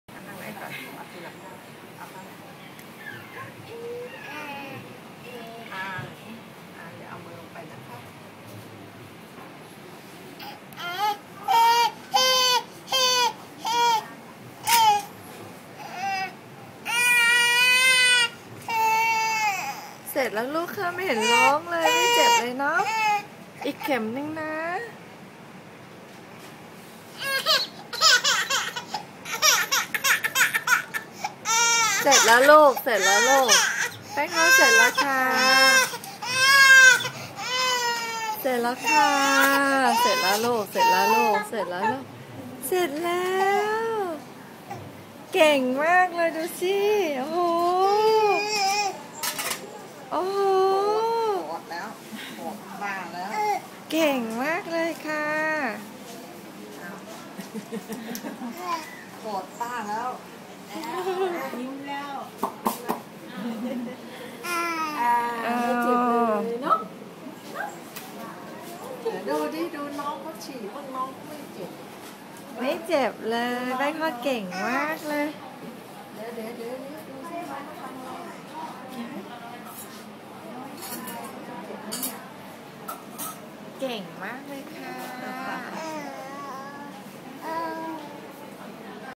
PaengTord's nine months vaccination
BIG girls don't cry.. And I'm a BIG girl now!!! ;D
My girl is so tough. She didn't cry much and managed to smile afterall... ^__^